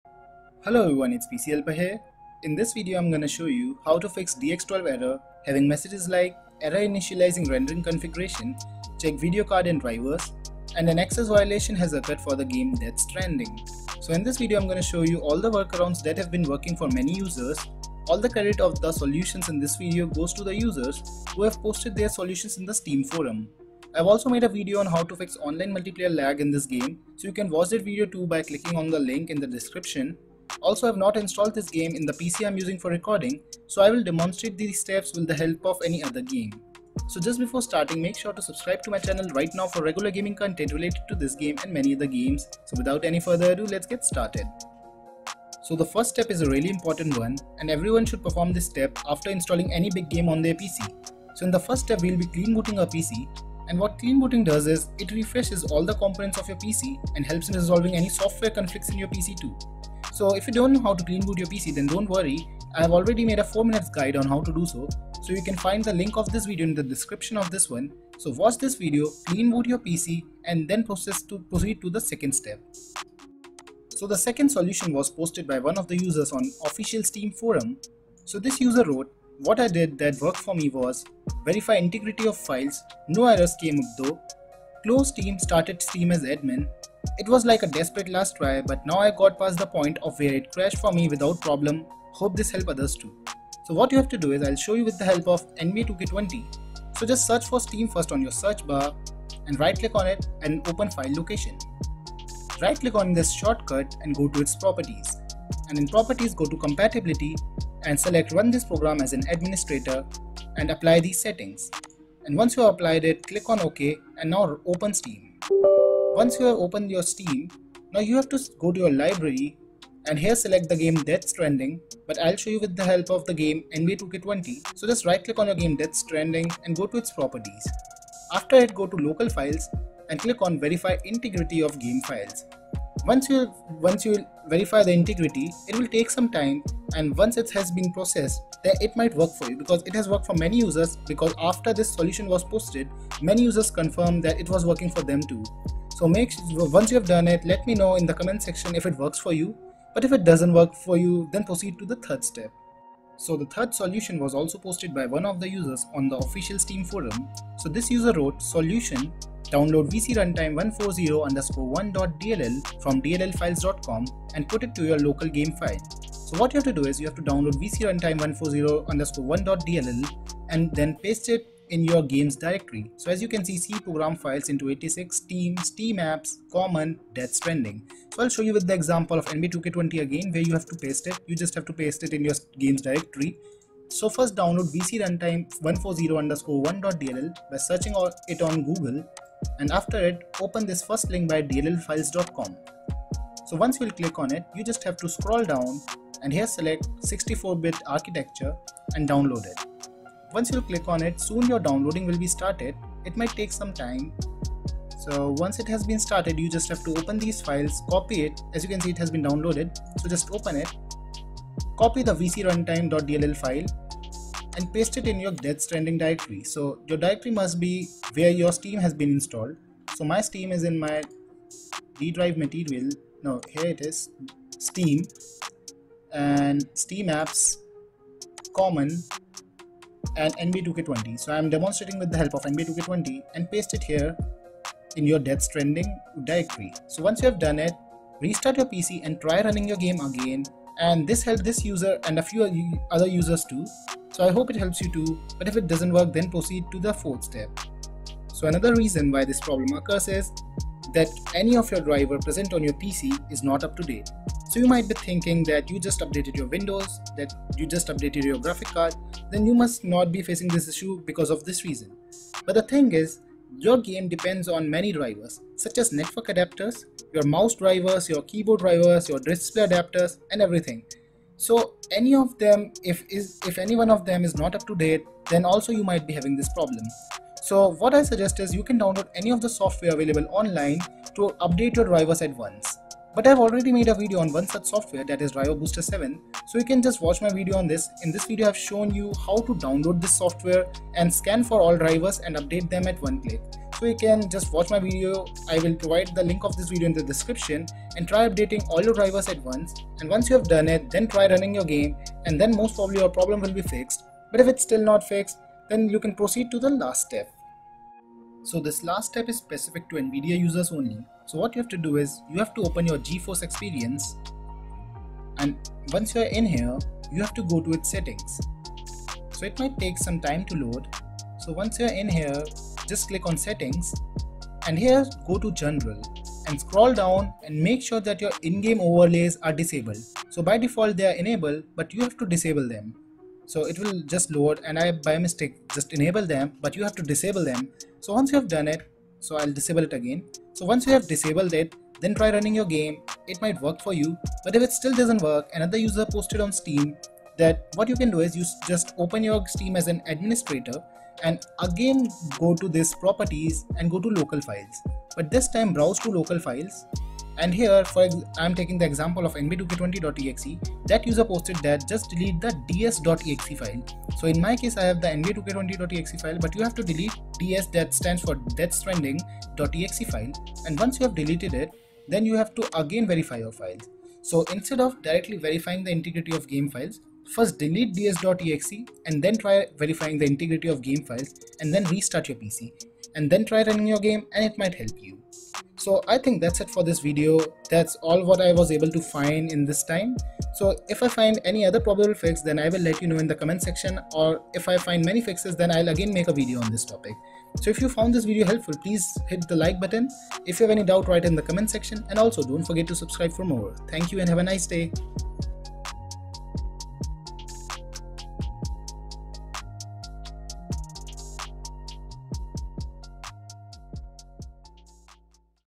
Death Stranding DX 12 Error FIXED | Access Violation (C0000005h) | Error initializing |Not Launching
In this video I' have shown how to fix Dx12-ErrorCode having messages like
Error initializing rendering configuration, check video card and drivers